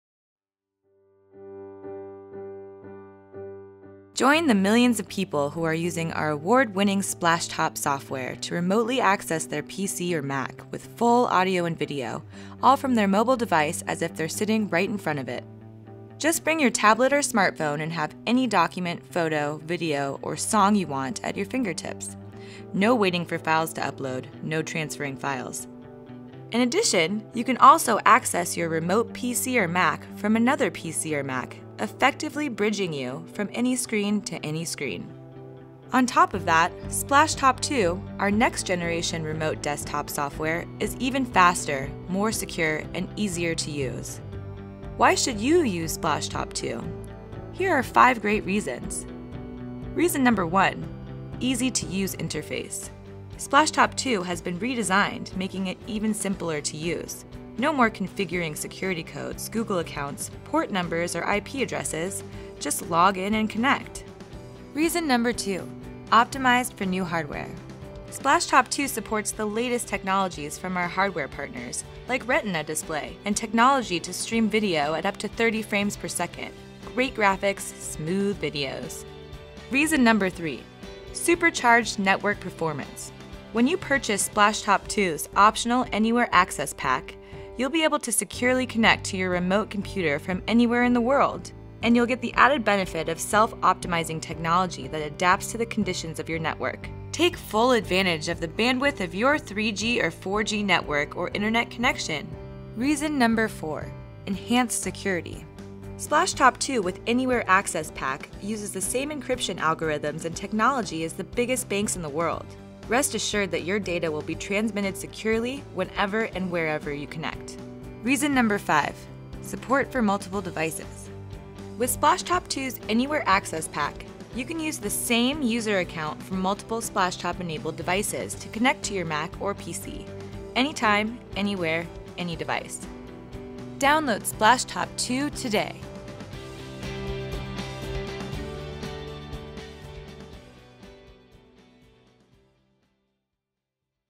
Splashtop Personal - Best Remote Desktop App with Great Audio and Video Support
Splashtop Personal is the next generation of our best-selling remote desktop app that lets you remotely access a PC or Mac from your iPad, tablet, smartphone, or any other mobile device. It's the easiest way to access all of your content from any device from anywhere with exceptional audio and video!  Compared to the original app, Splashtop Personal is faster, easier to use, and even more secure and reliable!